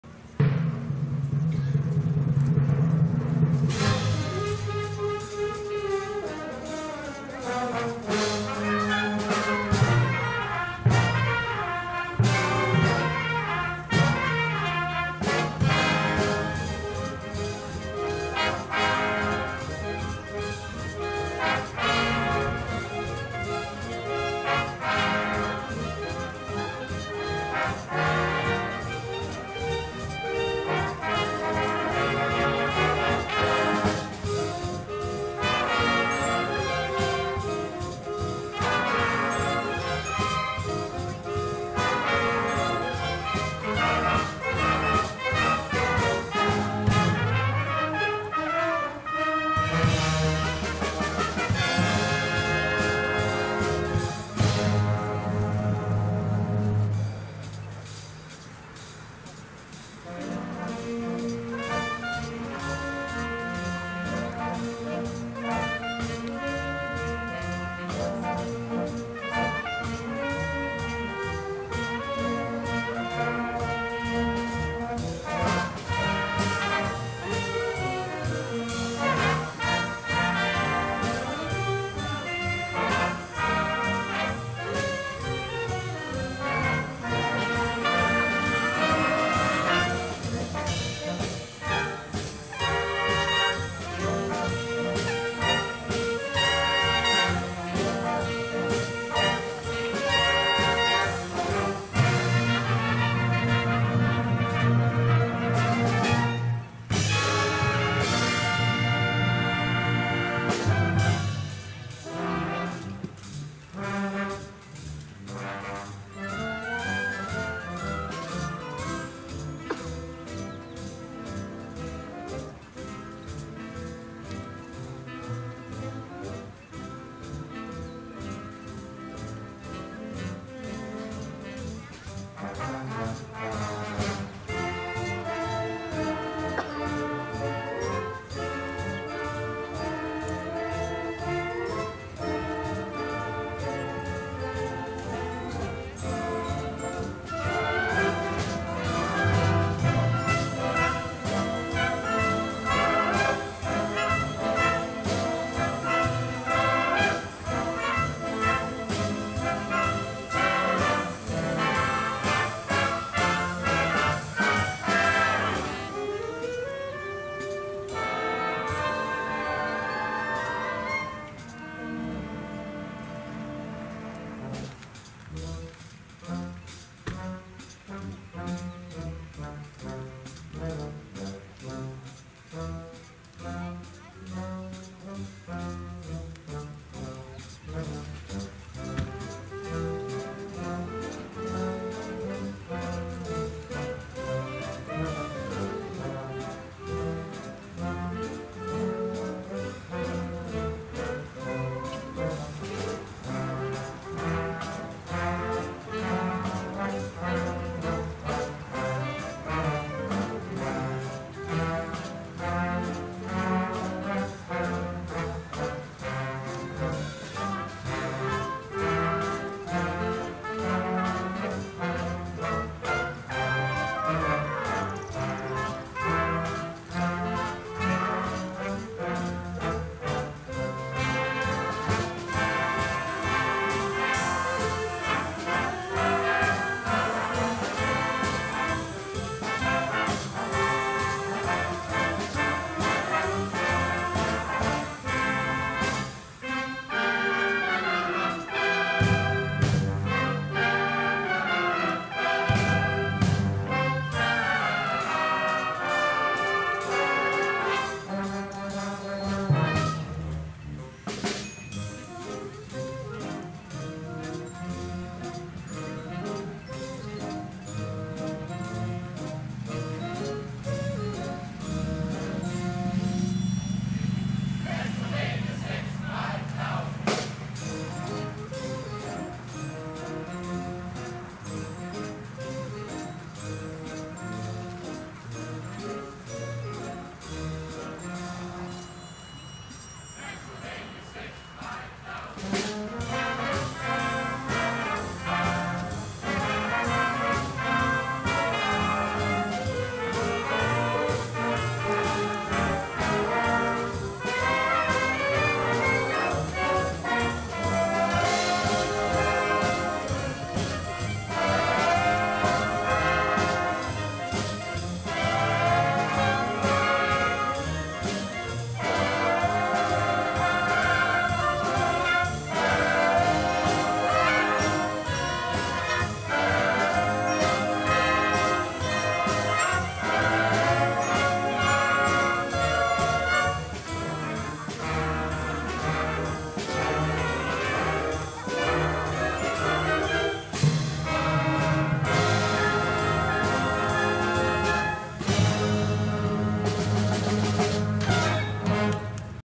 Glenn Miller in Concert
The Lakeshore Wind Ensemble perform in the 2022 Two Rivers Summer Concert series, the band is conducted by Ginger Van Ells. Videography by Jerry Galas.